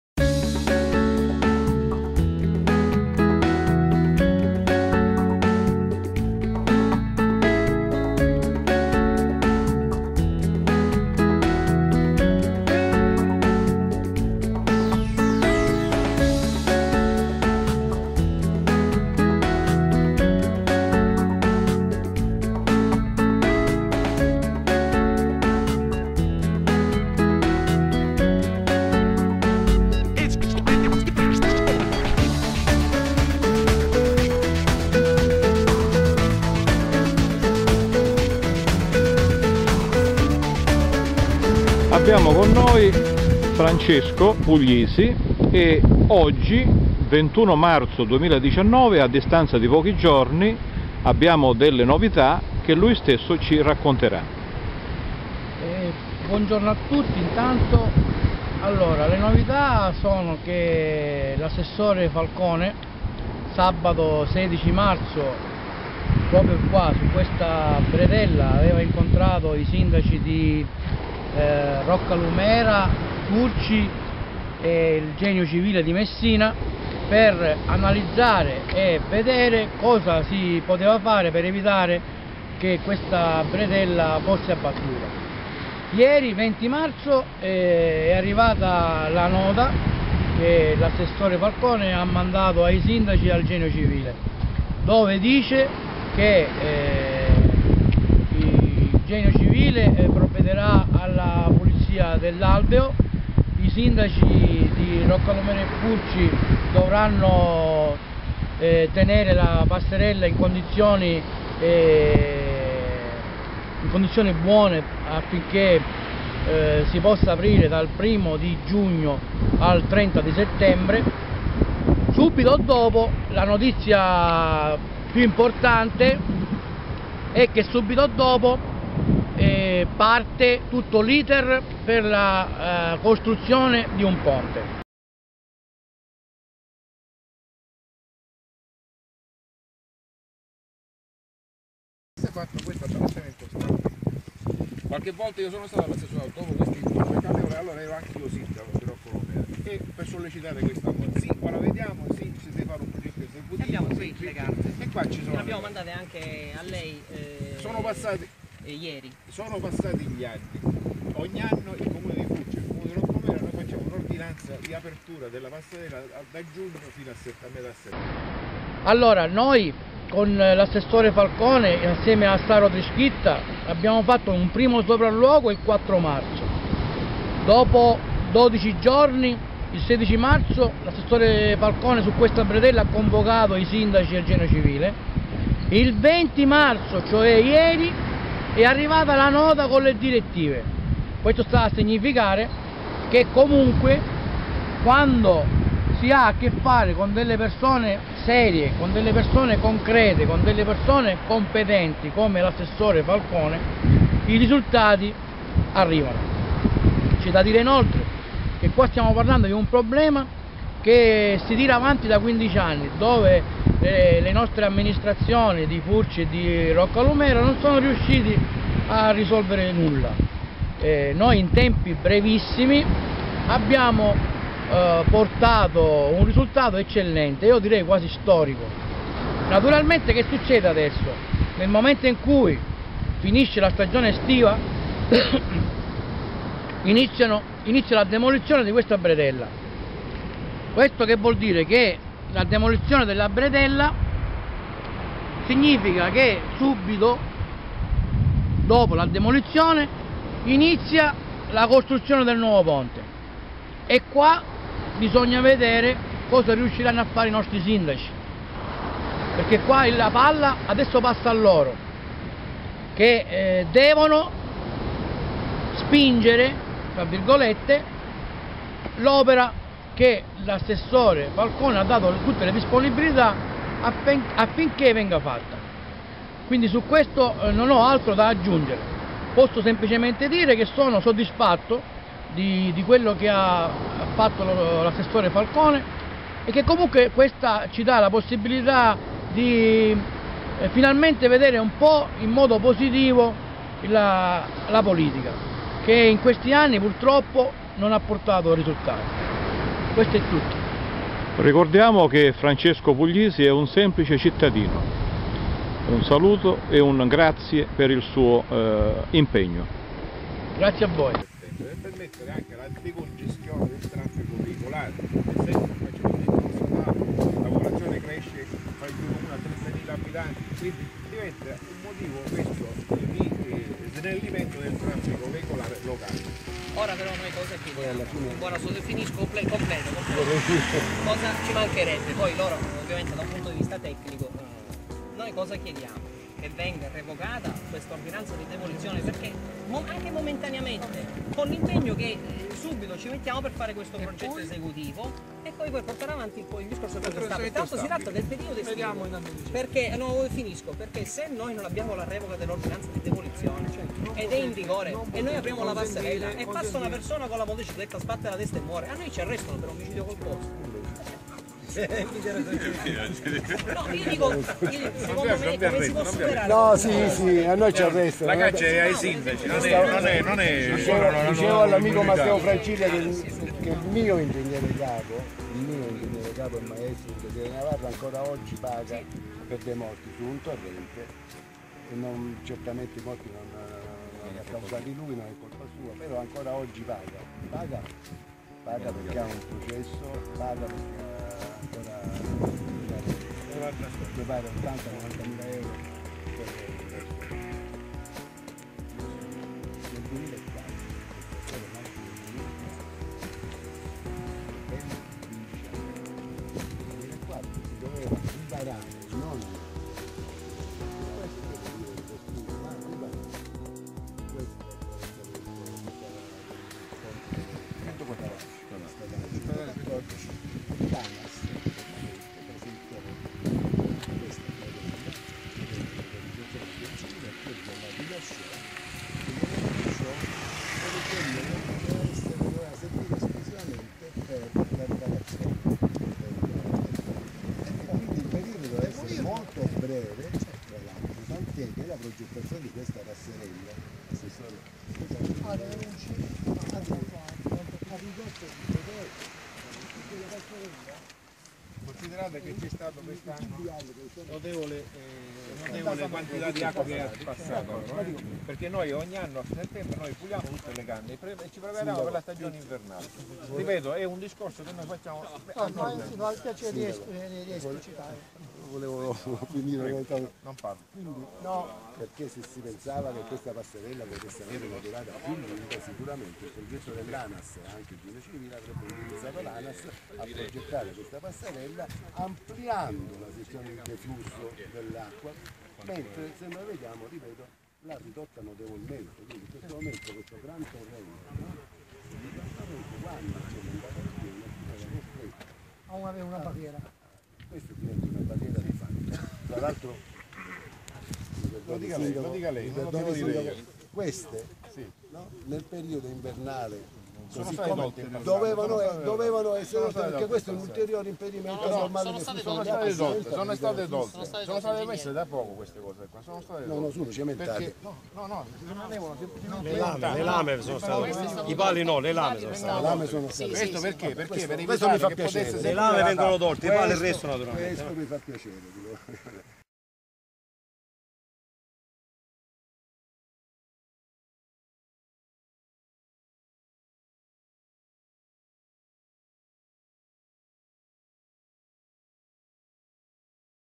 Francesco Puglisi. Le novità sulla bretella Roccalumera Furci (21.03.2019)
Roccalumera-Furci Siculo. Sabato 16 marzo 2019 l'incontro sulla bretella. Martedì 20 marzo i primi segnali dati dall'Assessore Regionale alle Infrastrutture Marco Falcone.